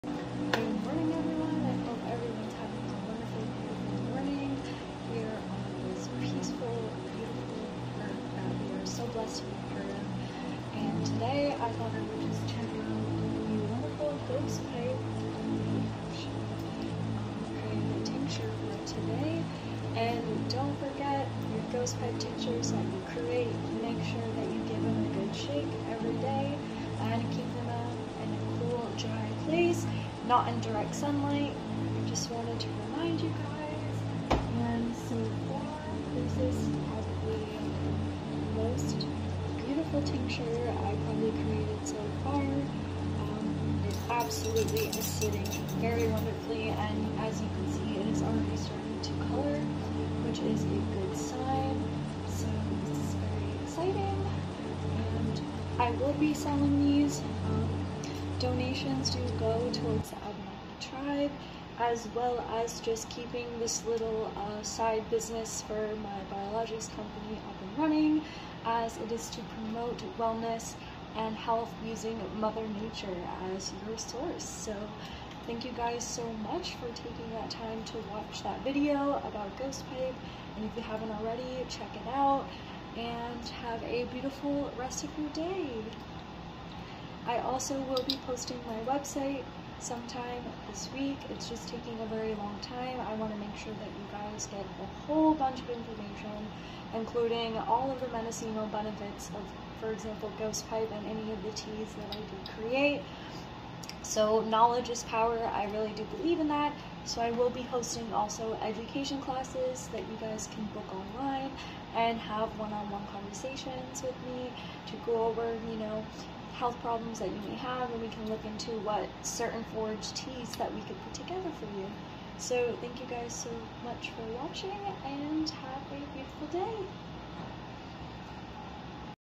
Ghost Pipe Check in! After 1 day it's already coloring beautifully 🌱
So blessed to be on a planet that gives such abundance for all! Please give when creating ghost pipe respect to native cultures as this has been passed down generations by Cherokee and Abenaki culture! Donations go to the Abenaki Tribe of New England please check out their website on how to support them!